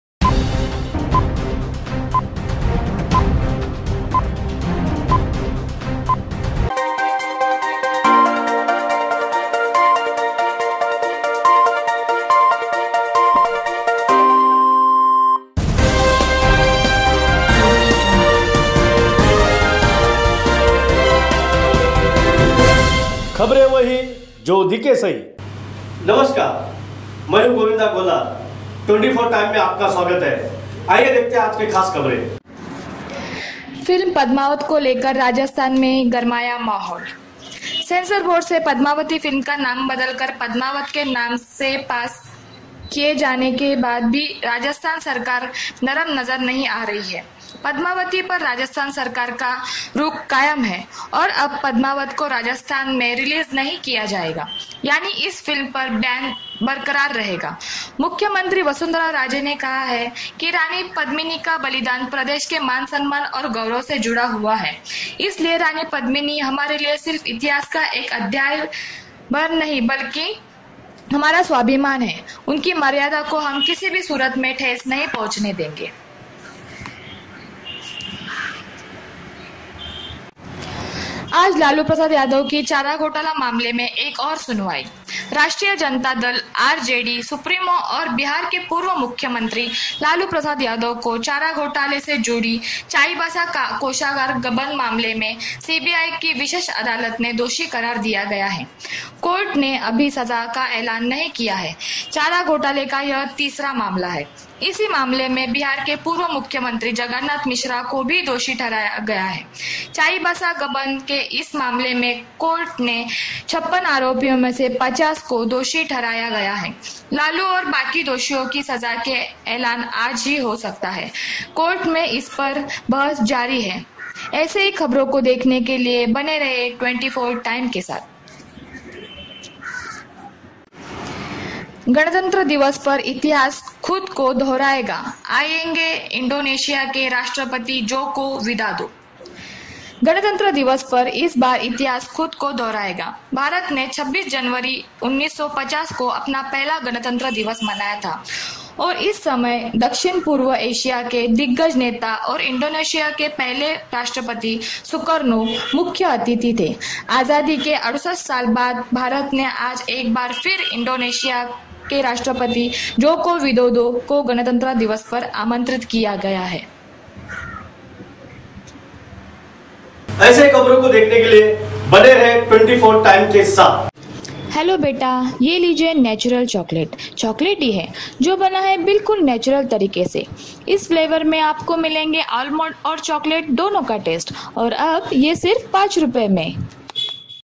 24 TIME NEWS (I) 24 JAN 2018
FILM PDMAVAT KE RELEASING PAR GRAMAYA MAHOUL-LALU KI RANCHI HIGHCOURT ME SUNWAYI- REPUBLIC DAY PAR DOHRAYA ITIHAAS.